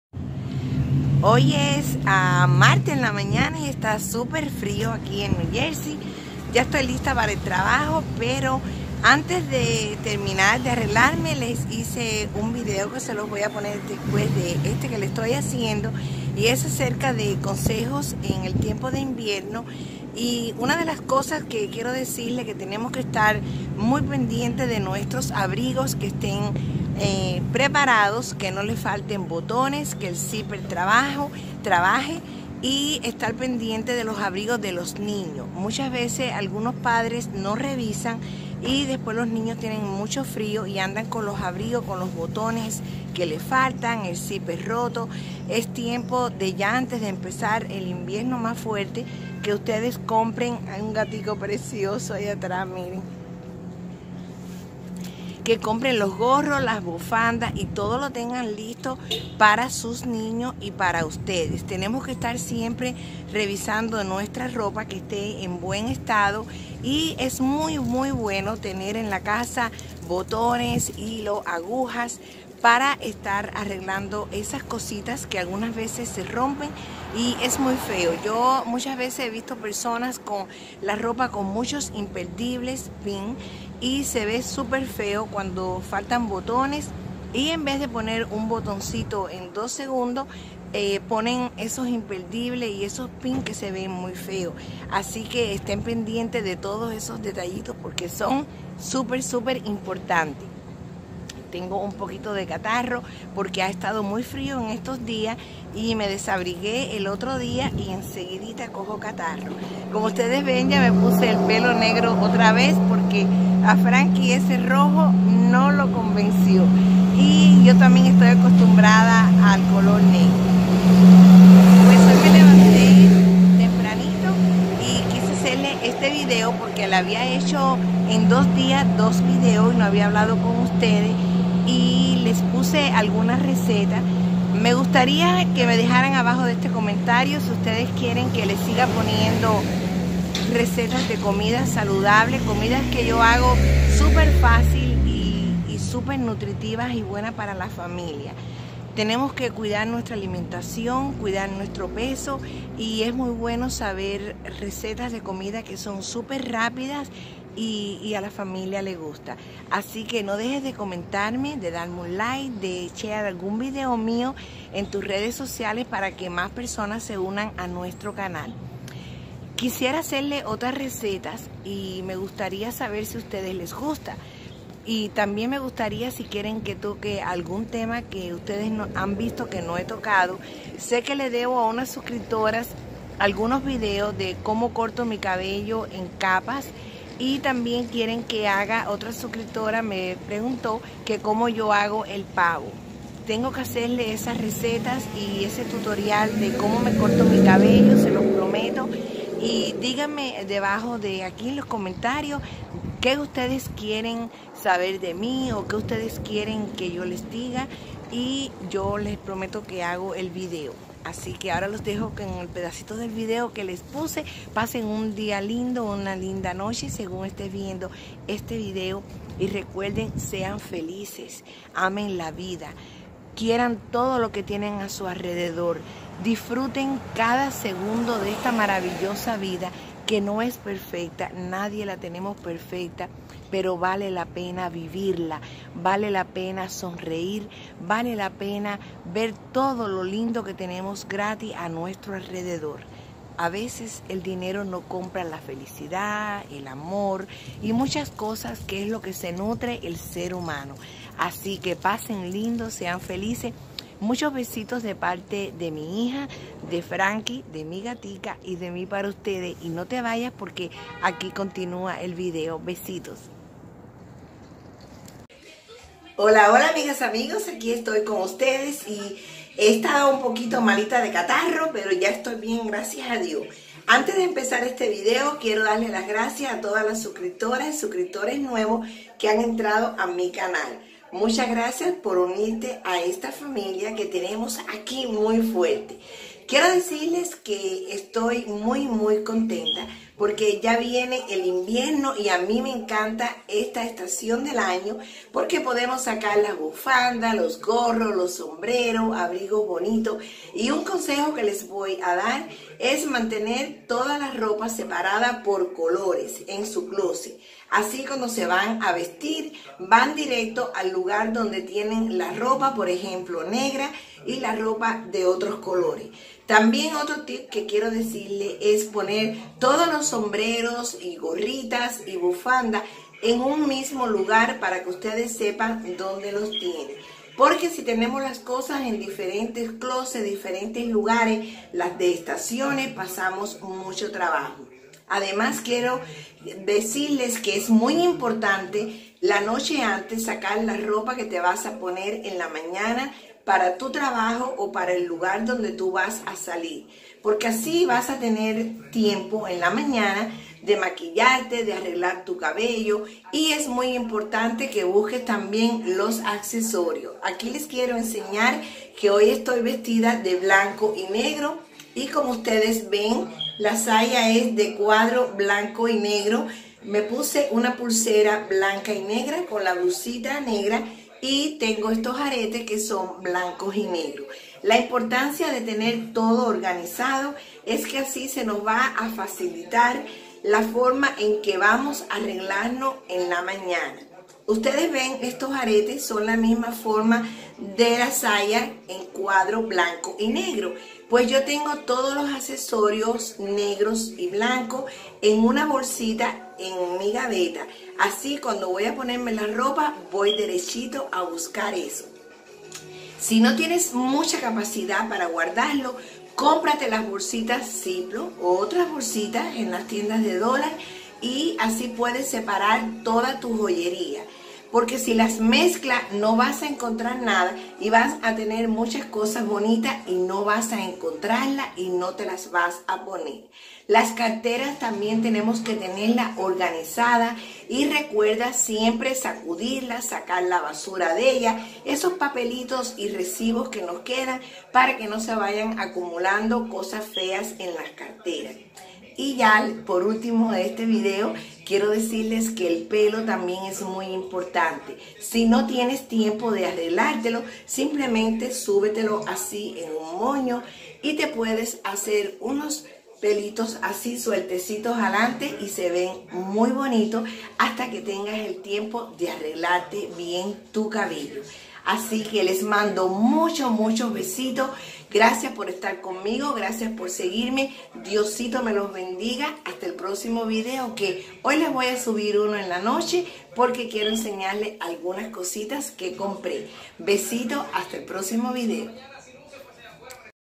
TIPS PARA EL INVIERNO + ORGANIZACION
amigas es muy importante organizar nuestra ropa y separarla por colores. pasen un dia o noche linda besitos